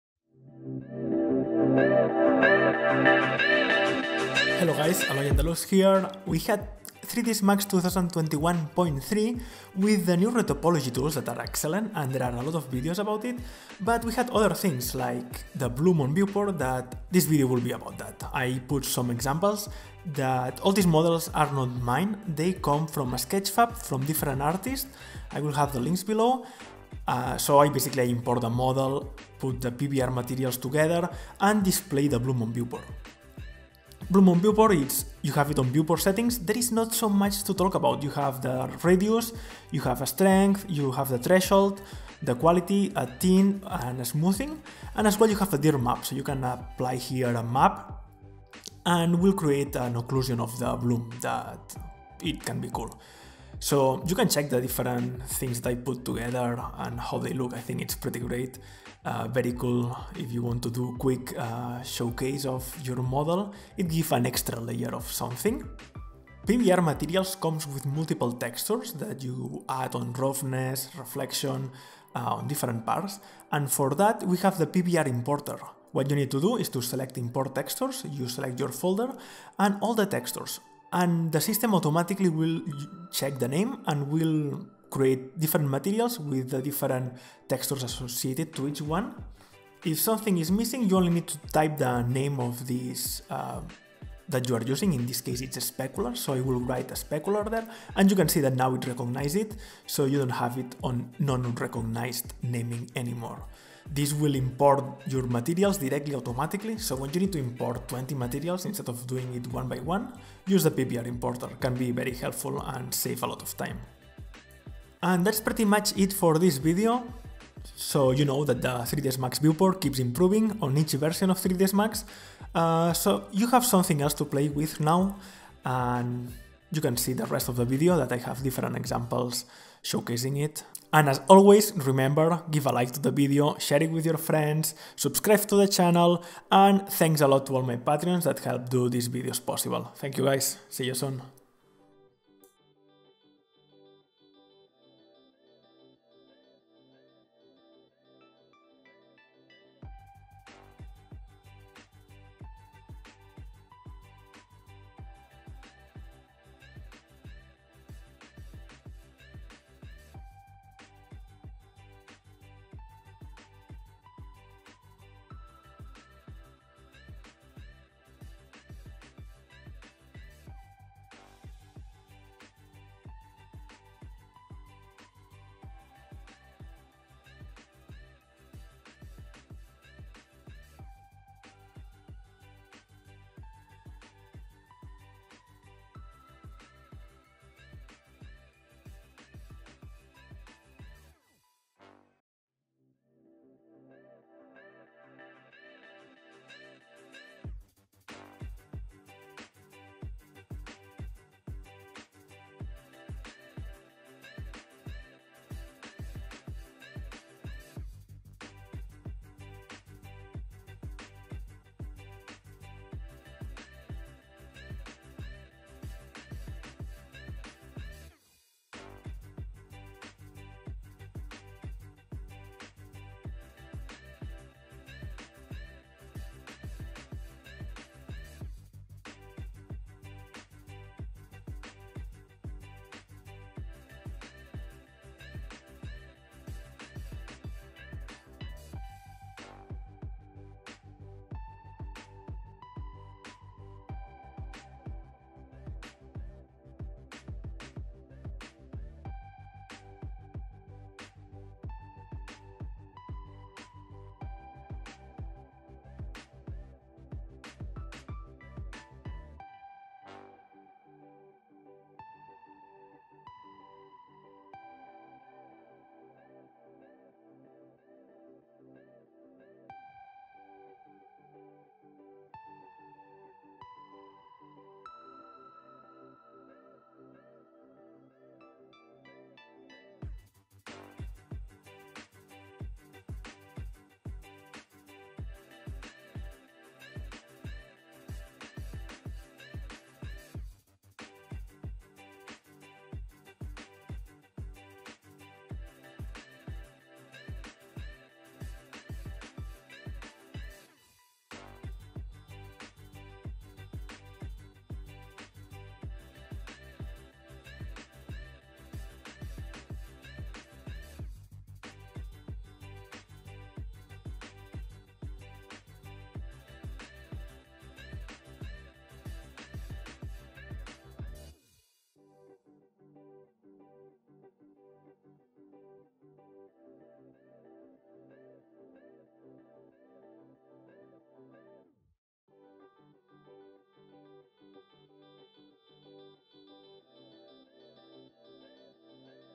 3dsMax 2021.3 // Bloom on viewport
We have bloom on viewport now in 3dsmax. its as easy as turn it "on", so this video is a series of examples put together, also showcasing  the new pbr importer.

None of this 3d models are done by me. from different artists.